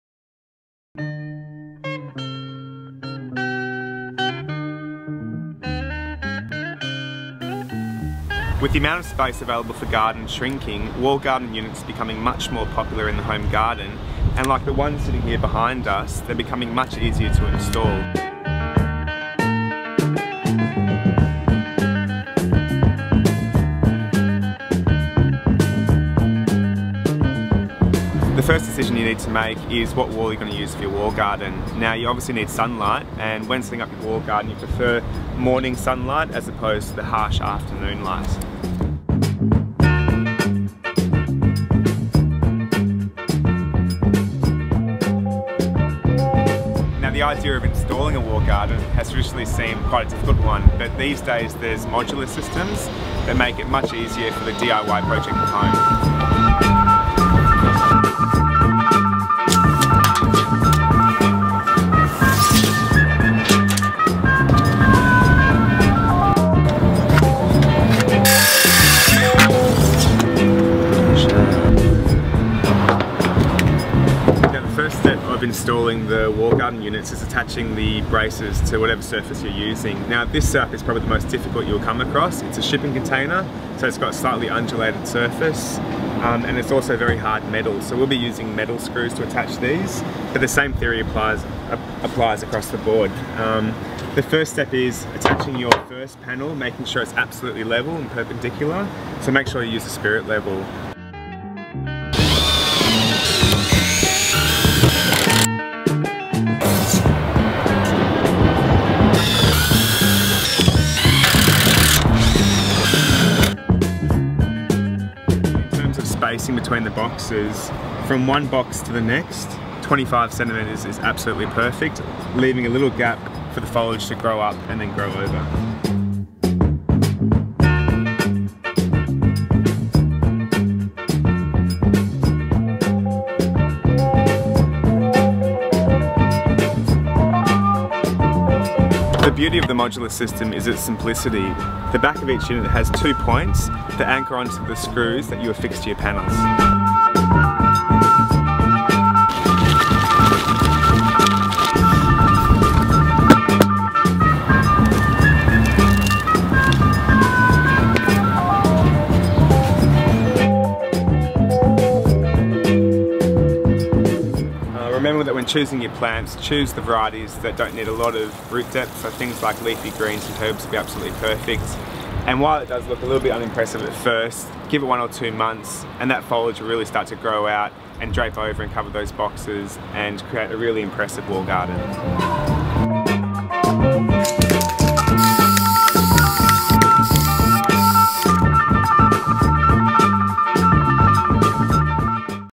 Vertical Living Wall Garden SetUp
Vertical Living Wall Garden. Setting up a wall garden is a lot easier than it appears. We take the mystery and the difficulty out of starting your own vertical garden.

Creating A Happy Sustainable Life!!